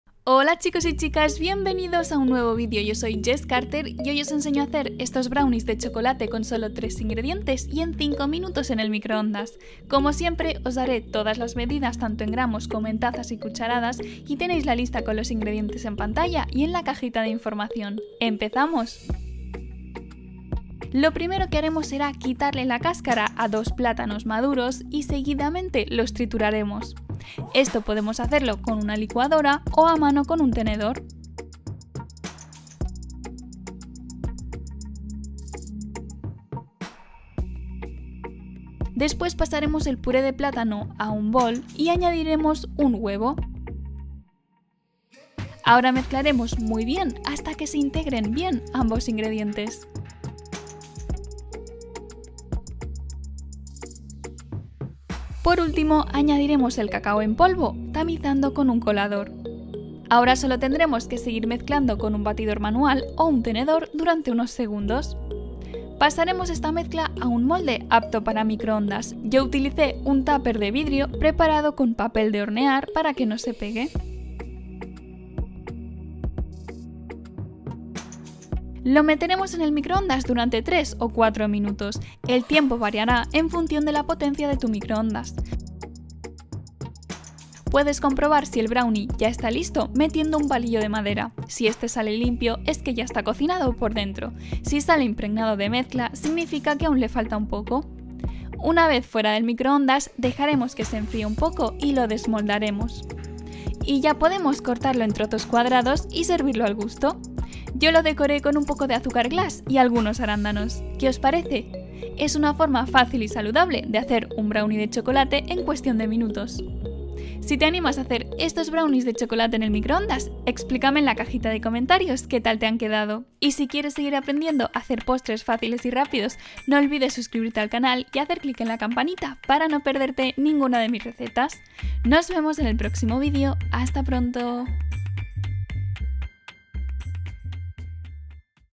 BROWNIE en MICROONDAS con 3 Ingredientes 🍫❤️ | en 5 minutos
❤️ ¿Quieres preparar un BROWNIE sin HORNO con plátano? En este vídeo te explico cómo hacer BROWNIES de CHOCOLATE en el microondas. ¡Descubre cómo hacer un BROWNIE en MICROONDAS con 3 ingredientes en solo 5 minutos!

🥣 INGREDIENTES para hacer el BROWNIE en MICROONDAS con 3 ingredientes:

🔸 2 plátanos
🔸 1 huevo
🔸 30 gr de cacao en polvo (1/4 de taza)

Jessie xx🔸